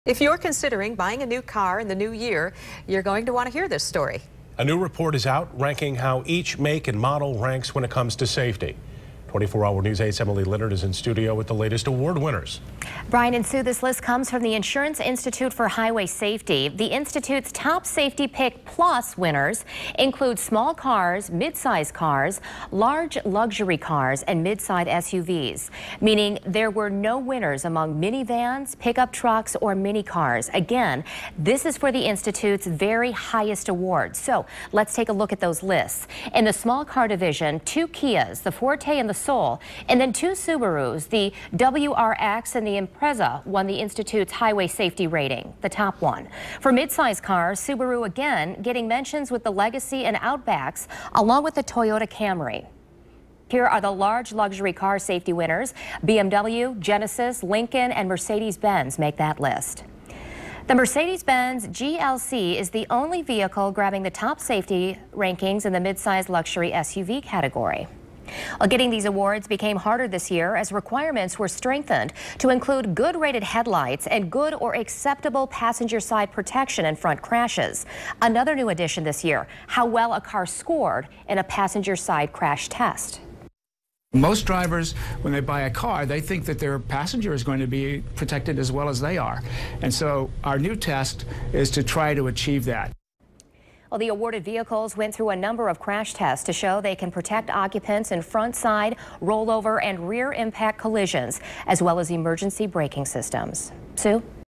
Insurance institute ranks vehicle safety
For those considering buying a new car in the new year, a new report lays out how each make ranks when it comes to safety. (Dec. 7, 2017)